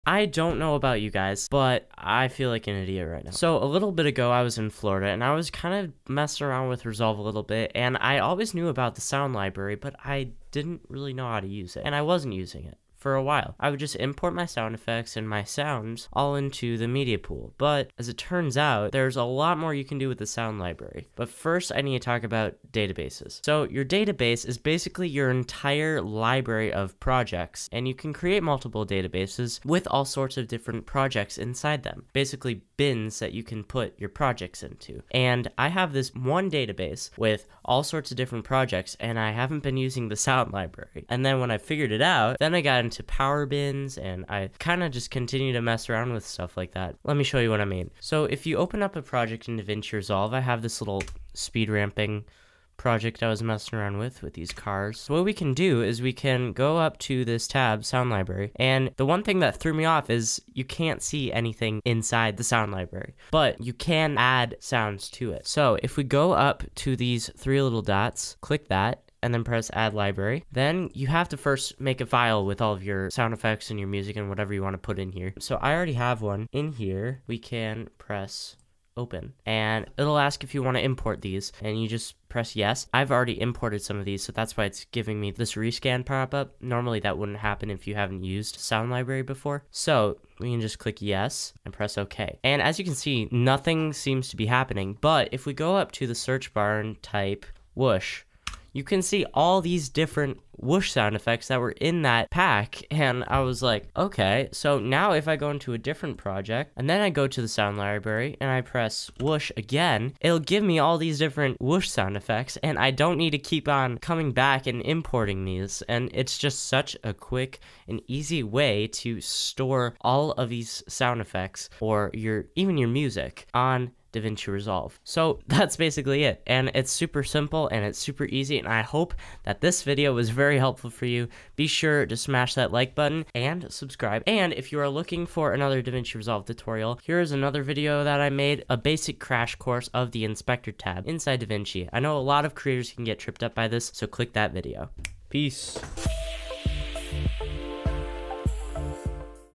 STOP Using the Media Pool for your SFX... (Davinci Resolve 18 Tutorial)
In this video, I'll be showing you a quick tip about Davinci Resolve that I didn't even know until a week ago! The Sound Library in the Edit page.
I'm kind of an idiot.

Chapters:
0:00 Expositional Monologue/Rant
0:21 Databases
0:50 The Fun Part
2:17 PLZ SUBSCRIBE